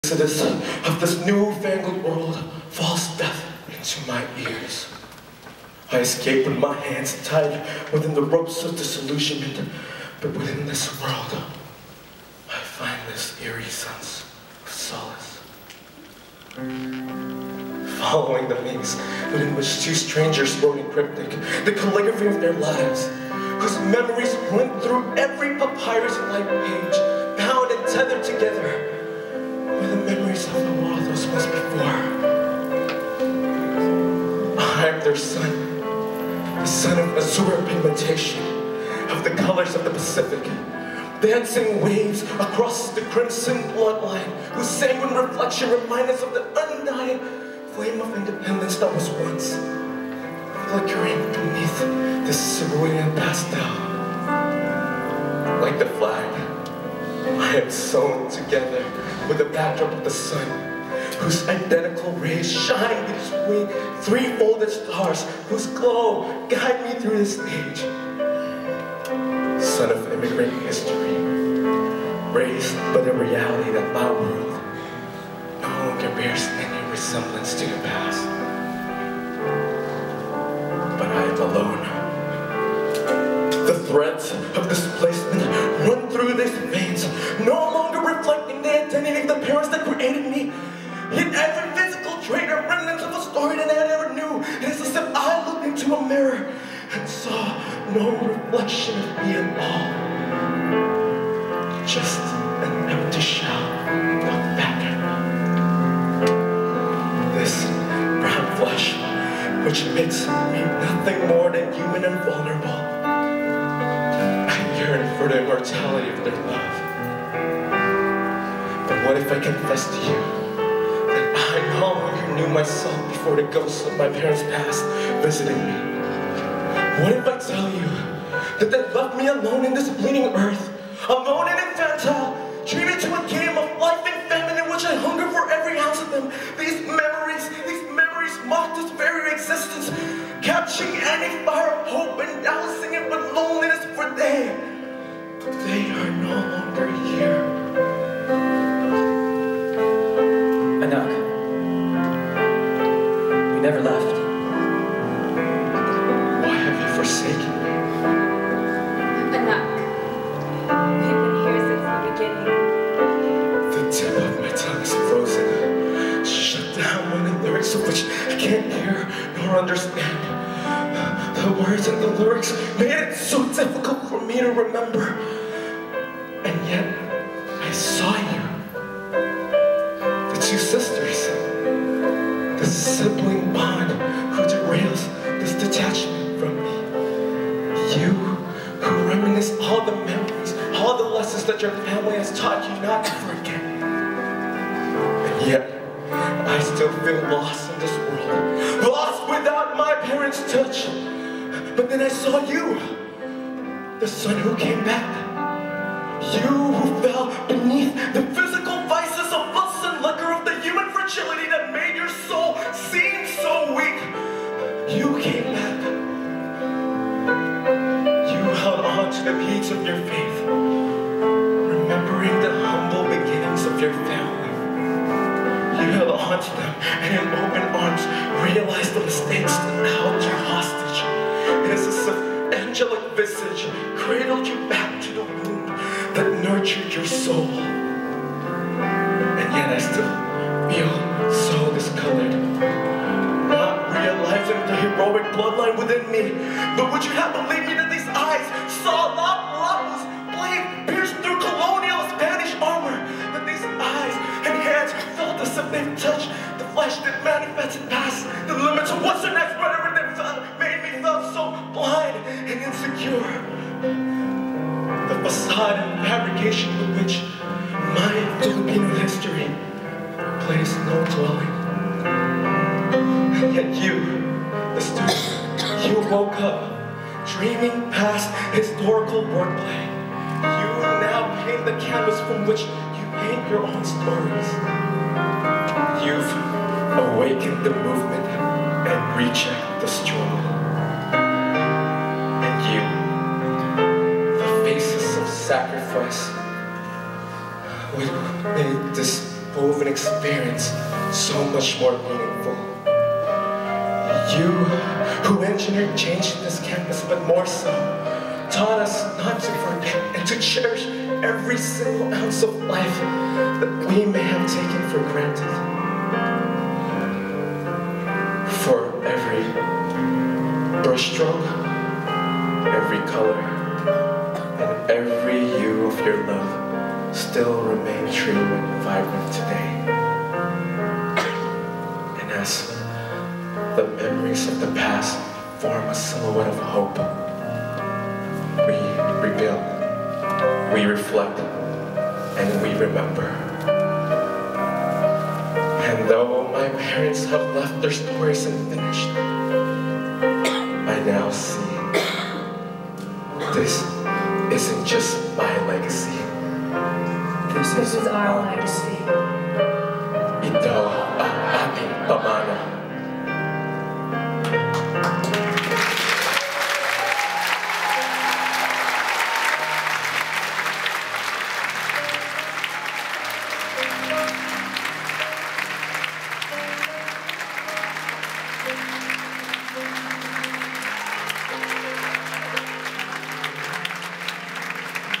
PCN 2012, Act 5, Rebirth
4.15.12 | Zellerbach Hall, UC Berkeley
University of California, Berkeley Pilipino American Alliance proudly presents the 36th annual Pilipino Cultural Night: Our Legacy, Ag Ating Pamana.
Act 5 | Rebirth | Spoken Word